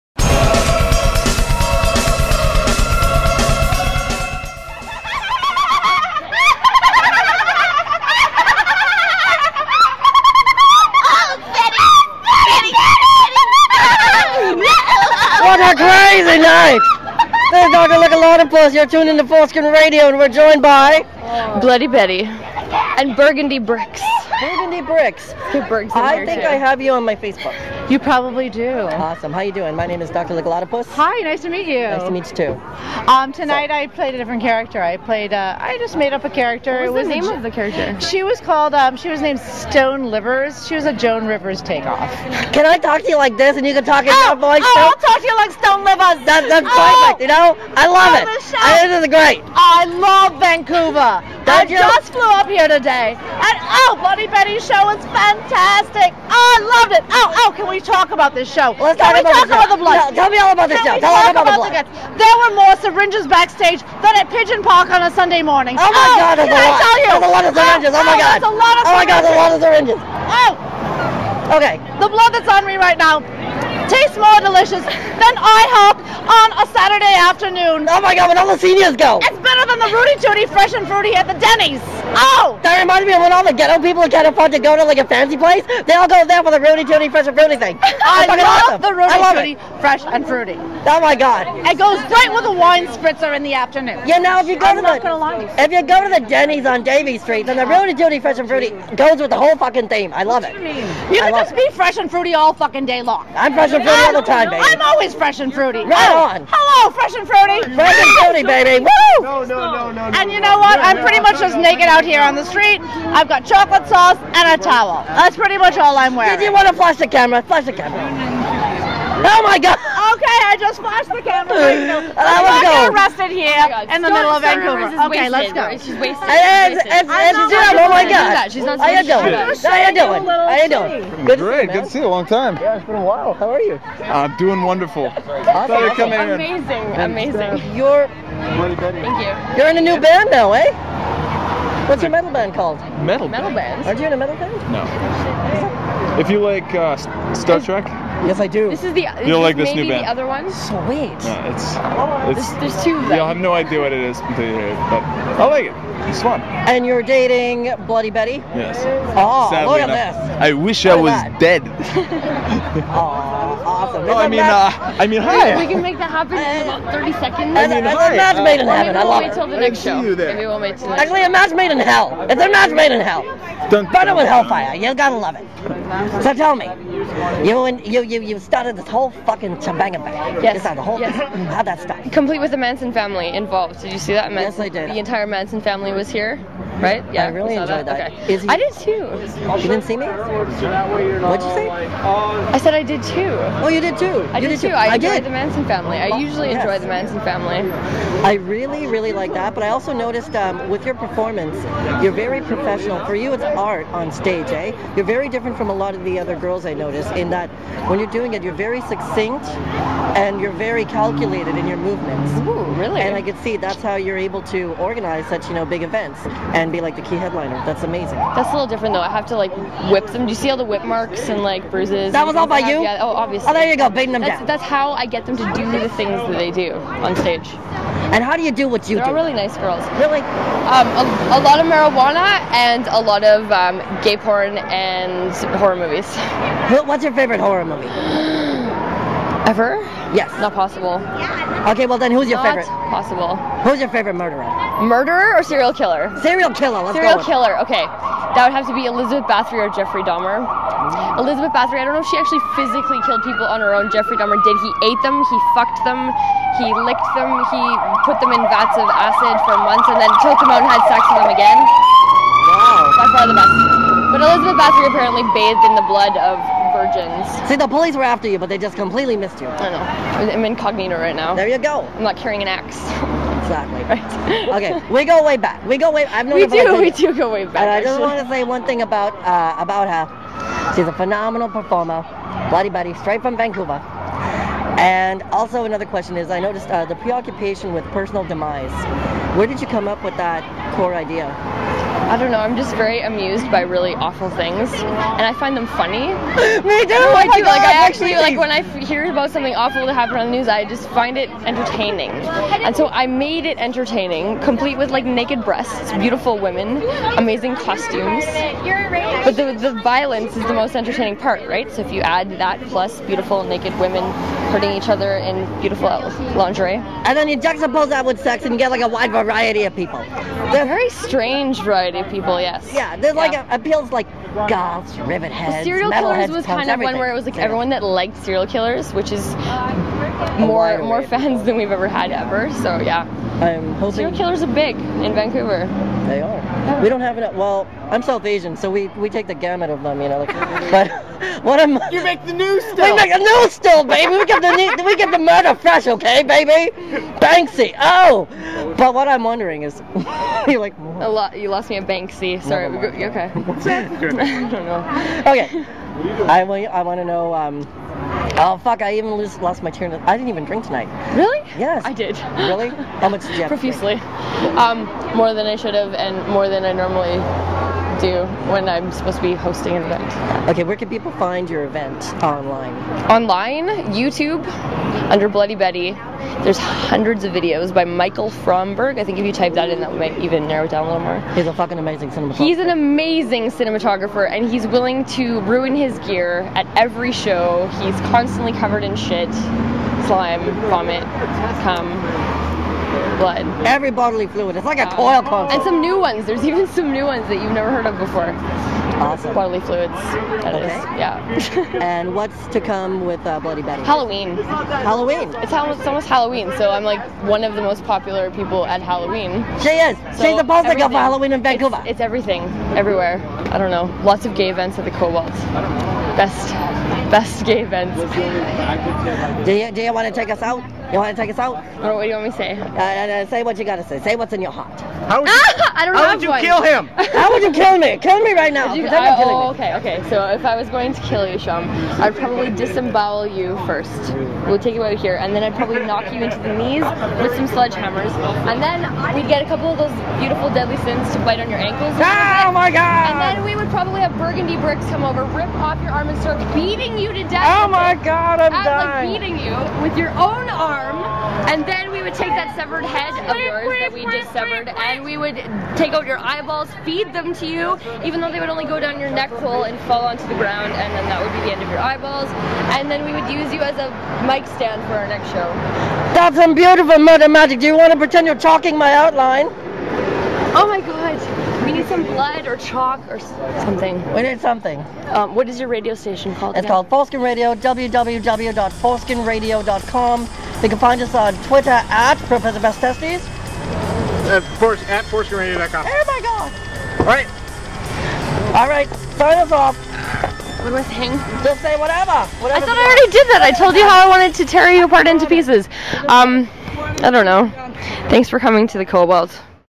Bloody Betty Interview [Foreskin Radio]
Interview with Bloody Betty and the Deadly Sins
with host Burgundy Brixx as "Stone Livers"
after gorelesque "Serial Killers Show"
Sept 7, 2011 at The Cobalt in Vancouver